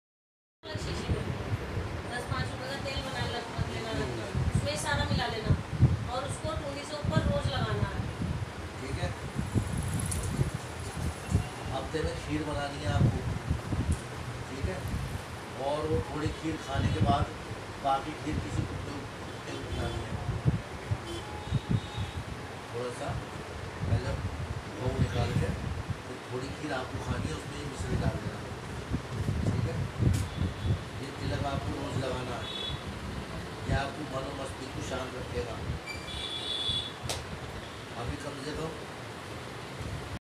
मेरठ से आए ललित की ऊपरी समस्या का समाधान करते हुए सोनी भगत जी गुरु मां संपर्क करें-8527233646
Video from sonididi bhagatji